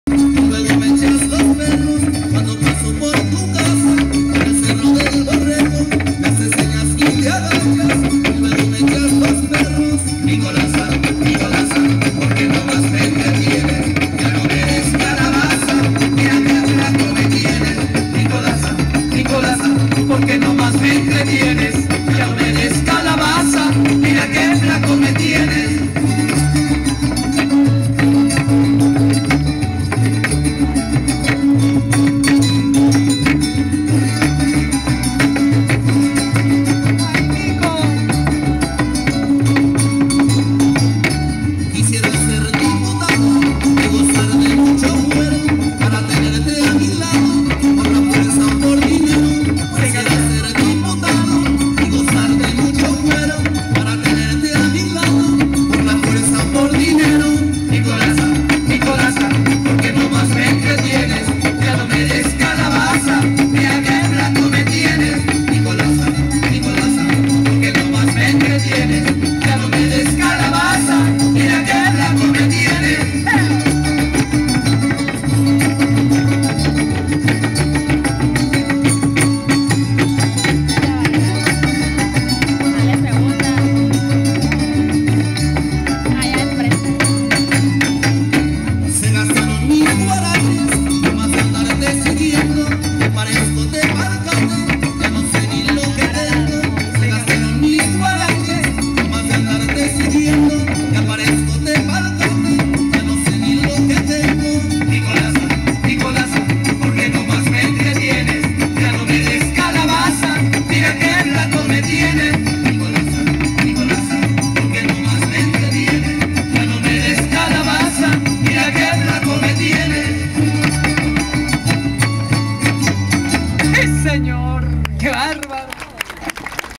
en el malecón de veracruz!!
en el puerto de veracruz apreciando su baile y su traje típico!! junio '07!